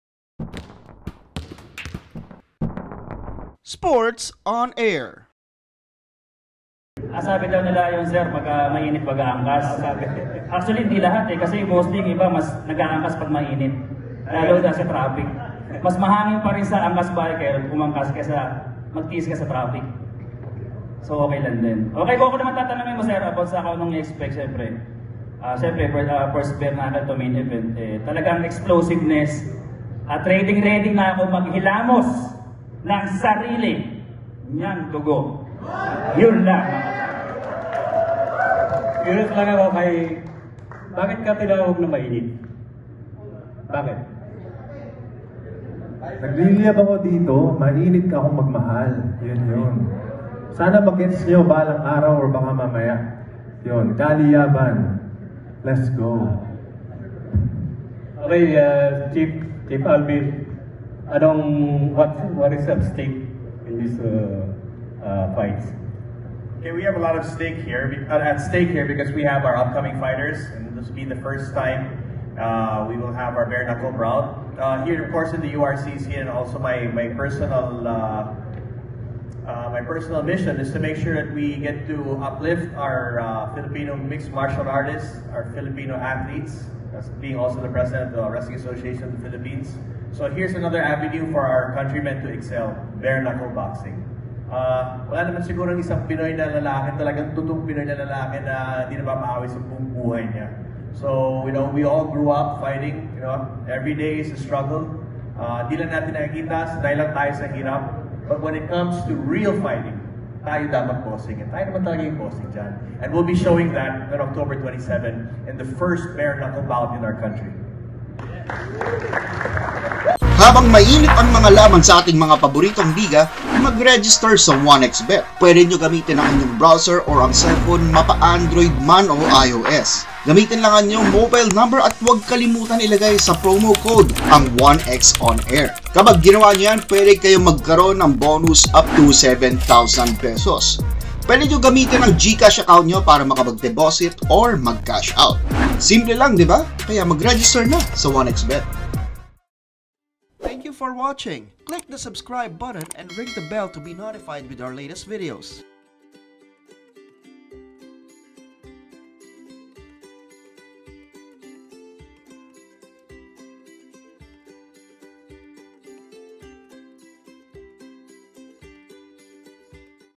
Angkas Biker HIHILAMOS NG DUGO ni Mr. Mainit sa kanilang bakbakan sa URCC
Map Soberano
Kenjhons Mr. Mainit
Bullet
Mammoth
Rolando Dy
Caloy Baduria